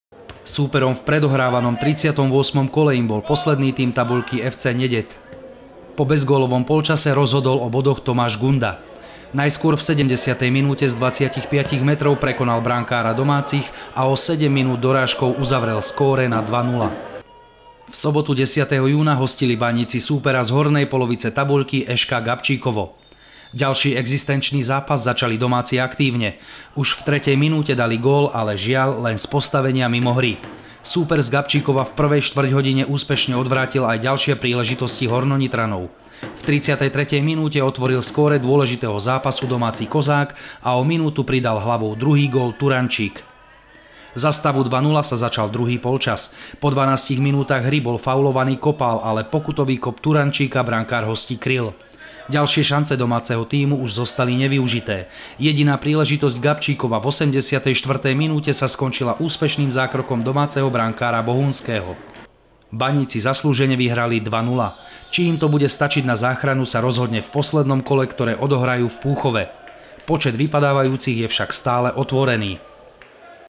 Rozhodne sa až po poslednom kole
V stredu 7.júna cestovali futbalisti FC Baník HN Prievidza a Handlová na juh Slovenska. Súperom v predohrávanom 38.kole im bol posledný tím tabuľky  FC Neded.

38.kolo
Neded - FC Baník HN Prievidza & Handlová  0:2 (0:0)
FC Baník HN : Bohunský - Šifalovič, Pekár, Filkorn, Belanec, Gunda, Kopál, Kováčik, Turančík.
Babašta, Kozák. Tréner: R. Slaný.
Rozh. : Havran, ŽK: Turančík (HN), 150 divákov
Góly : 70., 77. Gunda

V sobotu 10.júna hostili Baníci súpera z hornej polovice tabuľky – ŠK Gabčíkovo.

33.kolo
FC Baník HN Prievidza & Handlová – Gabčíkovo 2:0 (2:0)
FC BANÍK: Bohunský - Šifalovič, Pekár, Filkorn (85. Hlinka), Belanec (81. Sluka), Gunda, Kopál, Turančík (75.
Drozd), Halmeš, Babašta, Kozák. Tréner: Roman Slaný.
Rozh. : Papuča, 350 divákov. ŽK:  44. Belanec, 48. Gunda, 58. Turančík.
Góly : 33. D. Kozák, 34. A. Turančík.